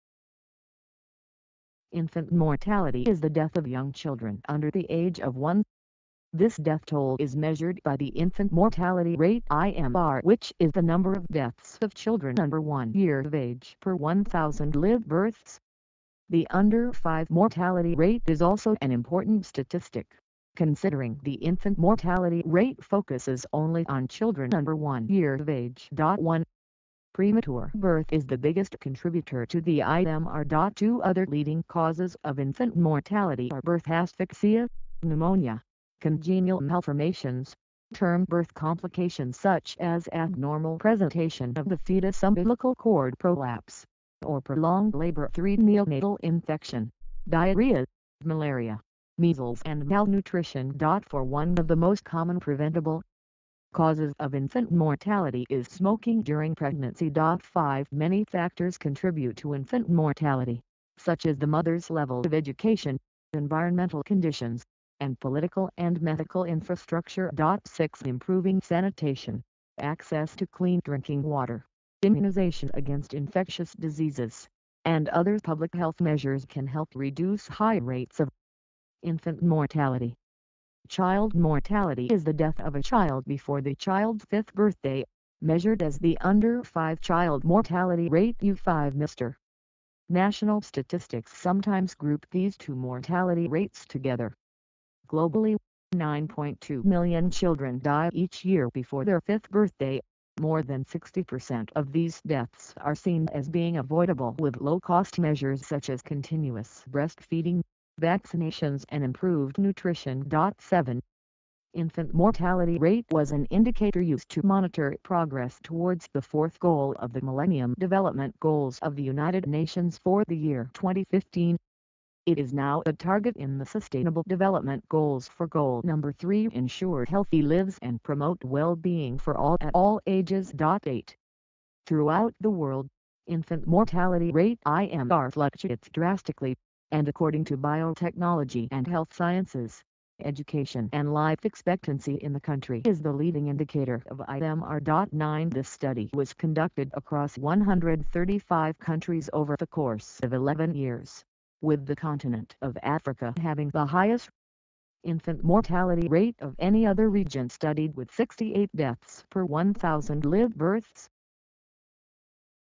What Is INFANT MORTALITY? INFANT MORTALITY Definition & Meaning
What is INFANT MORTALITY, What does INFANT MORTALITY mean, INFANT MORTALITY meaning, INFANT MORTALITY definition, INFANT MORTALITY explanation

Infant mortality is the death of young children under the age of 1. This death toll is measured by the infant mortality rate (IMR), which is the number of deaths of children under one year of age per 1000 live births. The under-five mortality rate is also an important statistic, considering the infant mortality rate focuses only on children under one year of age.[1]

Premature birth is the biggest contributor to the IMR.[2] Other leading causes of infant mortality are birth asphyxia, pneumonia, congenial malformations, term birth complications such as abnormal presentation of the foetus umbilical cord prolapse, or prolonged labor,[3] neonatal infection, diarrhea, malaria, measles and malnutrition.[4] One of the most common preventable causes of infant mortality is smoking during pregnancy.[5] Many factors contribute to infant mortality, such as the mother's level of education, environmental conditions, and political and medical infrastructure.[6] Improving sanitation, access to clean drinking water, immunization against infectious diseases, and other public health measures can help reduce high rates of infant mortality.

Child mortality is the death of a child before the child's fifth birthday, measured as the under-5 child mortality rate (U5MR). National statistics sometimes group these two mortality rates together. Globally, 9.2 million children die each year before their fifth birthday; more than 60% of these deaths are seen as being avoidable with low-cost measures such as continuous breast-feeding, vaccinations and improved nutrition.[7]

Infant mortality rate was an indicator used to monitor progress towards the Fourth Goal of the Millennium Development Goals of the United Nations for the year 2015. It is now a target in the Sustainable Development Goals for Goal Number 3 ("Ensure healthy lives and promote well-being for all at all ages").[8]

Throughout the world, infant mortality rate (IMR) fluctuates drastically, and according to Biotechnology and Health Sciences, education and life expectancy in the country is the leading indicator of IMR.[9] This study was conducted across 135 countries over the course of 11 years, with the continent of Africa having the highest infant mortality rate of any other region studied with 68 deaths per 1,000 live births.
Source: Wikipedia.org